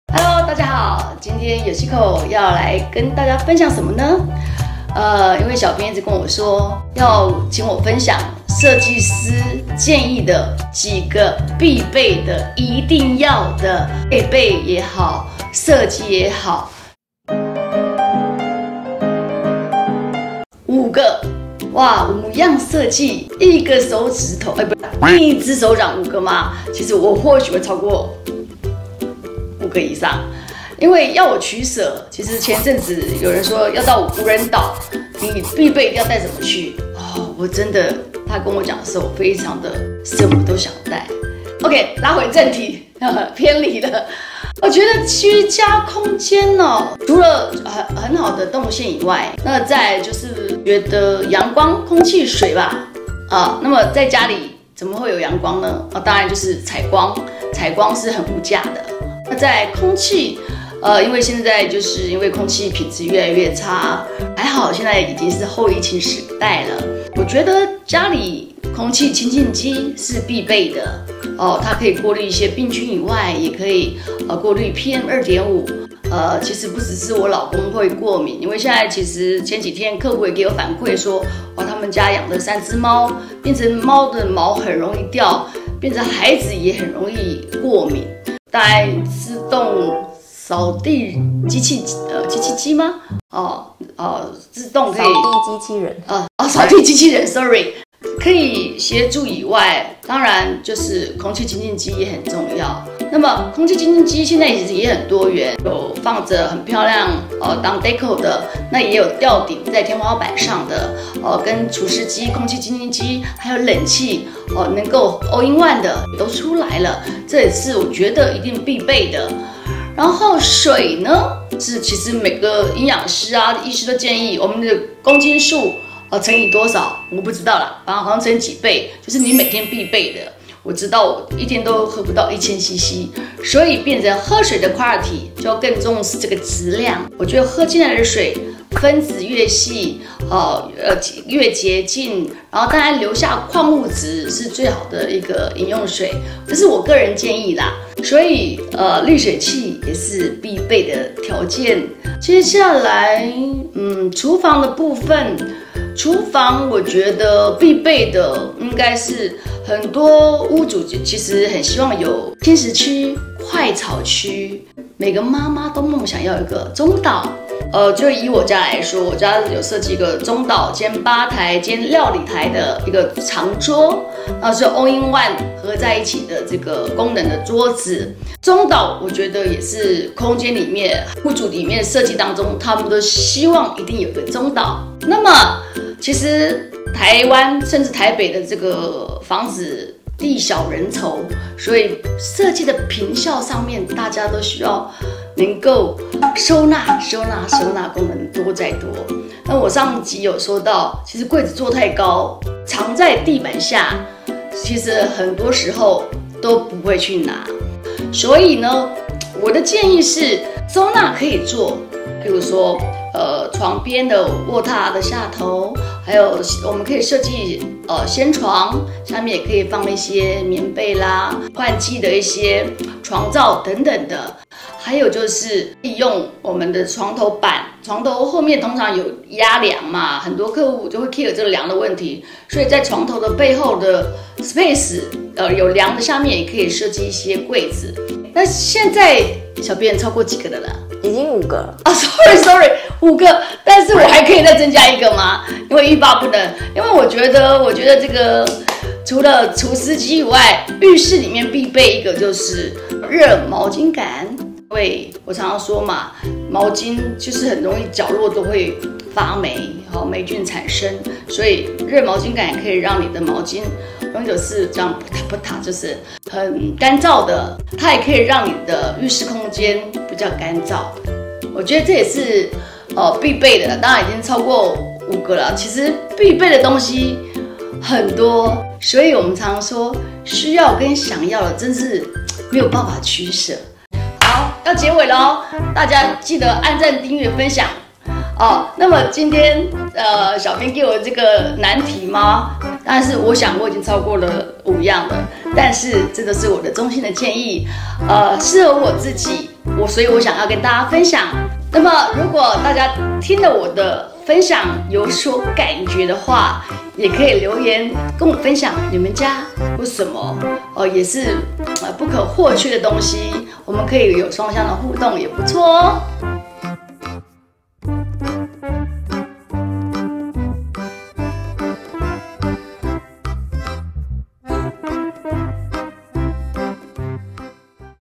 【設計師真心話】如果被丟到無人島要帶什麼? | YOCICO的五樣精選設計有哪些? | 設計師精選設備
把YOCICO丟到無人島??
不是啦~如果今天只能帶走五樣設計
設計師不可或缺的設計是什麼呢?

陽光、空氣、水都太重要了
該怎麼取捨真是太為難啦!!

想知道YOCICO的精選設計嗎
千萬不能錯過今天的影片喔!!
程翊官方LINE ID: tei1688
地址:台北市內湖區金湖路363巷67弄59號